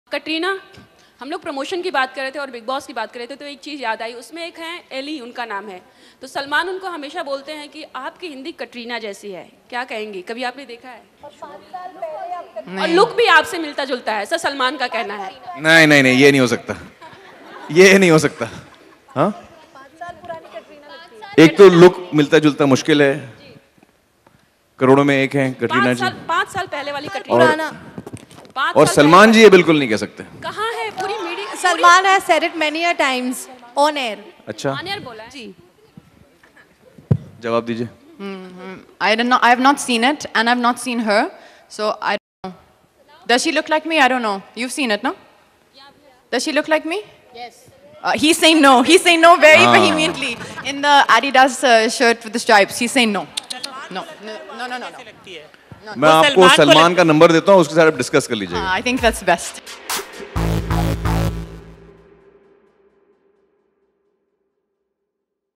Katrina Kaif REACTS on Salman Khan's Bigg Boss 7 contestant Elli Evram
Bollywood hottie Katrina Kaif spoke about Bigg Boss 7 contestant Elli Evram and Salman Khan's fondness to the beauty at the Dhoom 3 song launch. Watch the video to find out how Katrina responds to this googly question.